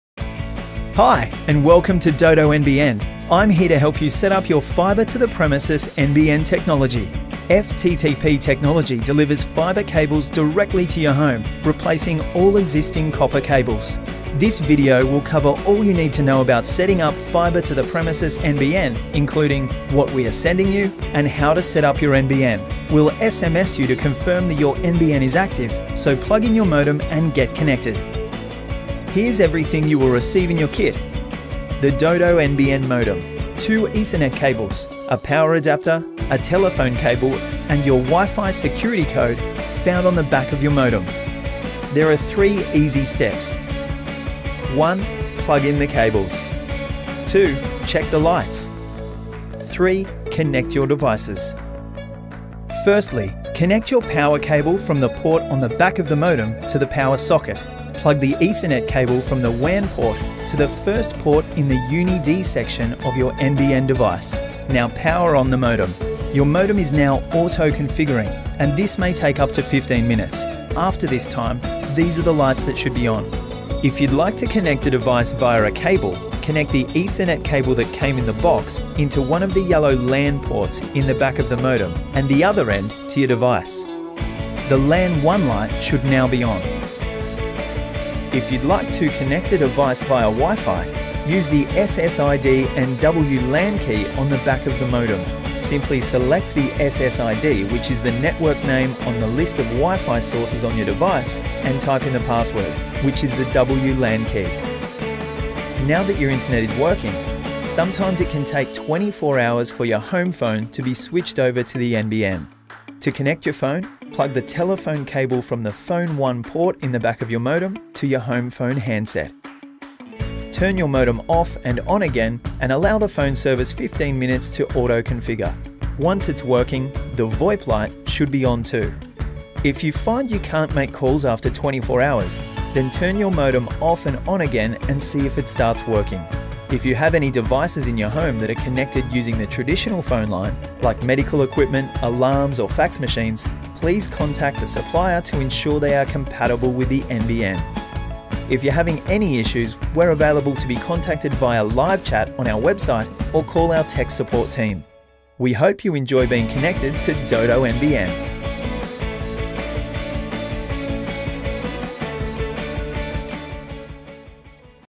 Dodo Modem Set-up Guide- FTTP NBN Technology
Here's a quick video showing you how to set-up your Dodo NBN if you have Fibre to the Premises (FTTP) technology at your home.

Need a bit of extra help to get connected?

We've included instructions in your modem pack you received with your modem.

Alternatively, you can LiveChat with us online, so jump over to our to chat with our team from 9am-10pm AEST.